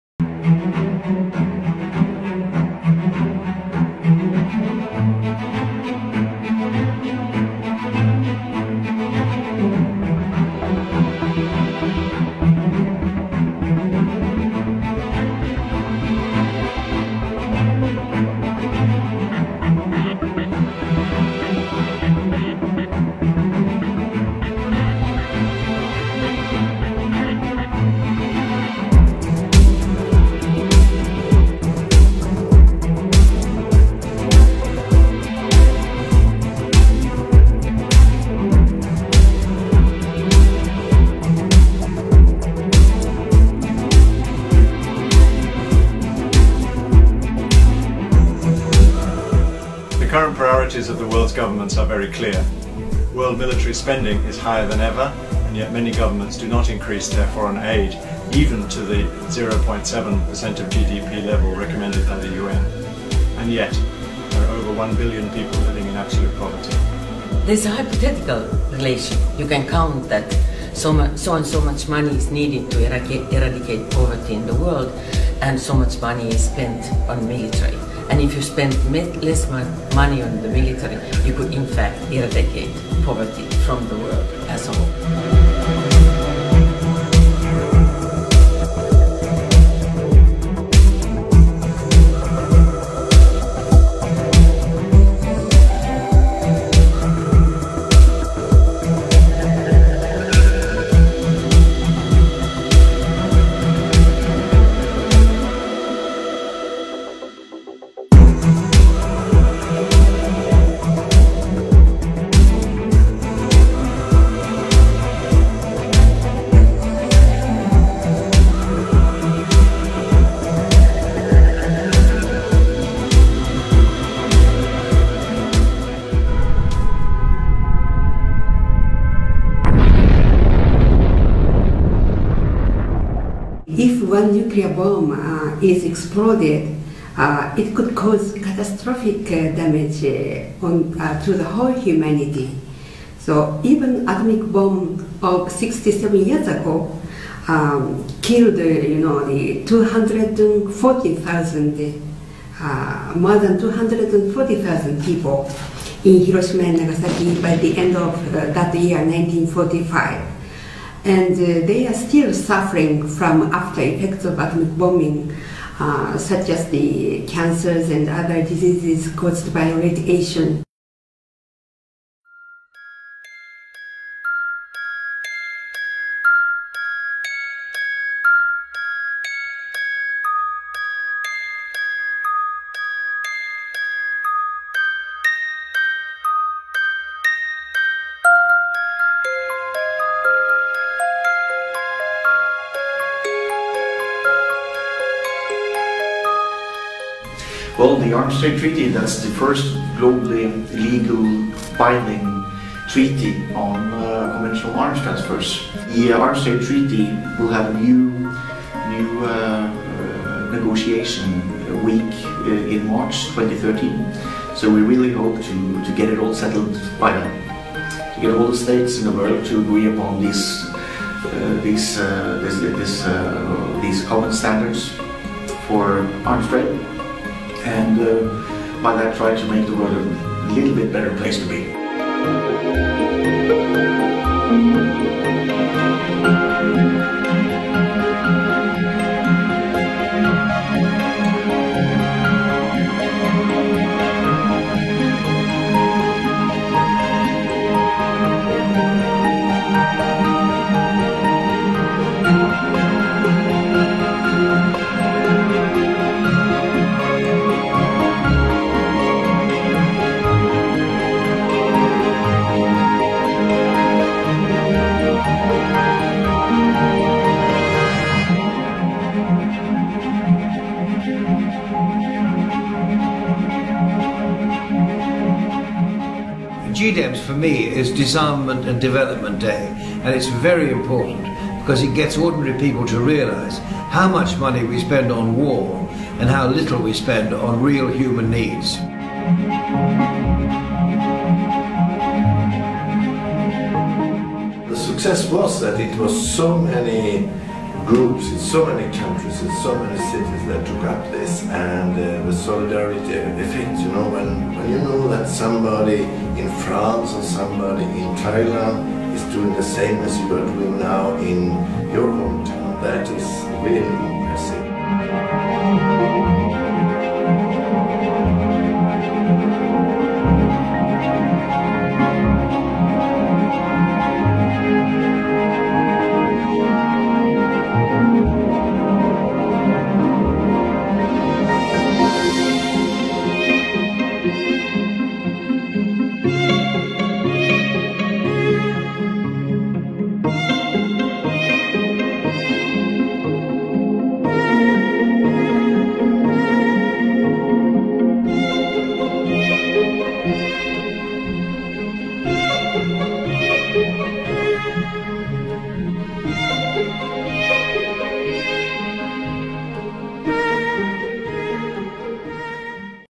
GDAMS 2015
"The World is Over-Armed and Peace is Underfunded" Ban Ki-Moon
In 2013, global military spending surged to an all-time high of US$1.75 trillion. Given the numerous crises facing the planet – the environment, the economy, epidemics, mass poverty – is this the right priority for the use of public money? We say no: a massive reallocation is needed. There are thousands of organizations and millions of individuals who support this point of view – what is needed is a serious mobilizing effort to make it visible.
The GDAMS movement would like to persuade governments all over the world to reallocate the global military spending to urgent human needs. Apart from organizing and participating in local events, we urge you to use social media as well.